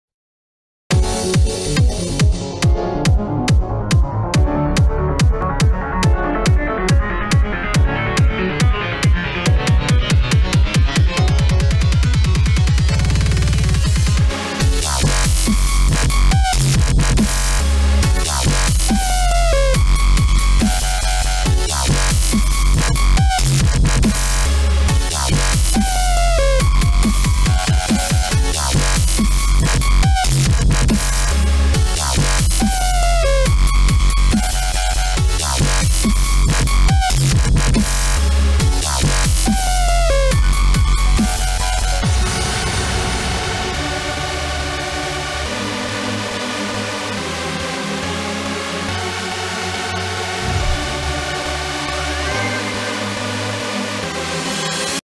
(Full Preview) Natural Chromatic By: GalaxyGaming, DoubleSsymbol, Strix and HexagonHavoc
This is an unoffical interquel of Sharp Minor and Flat Major (by Giron/ASonic & JakeN2 respectively), there is going to be 2 versions of this level, 1.9 deco and 2.1 deco, so hope you guys like this level :)

Thumbnail by: ViralX

nashe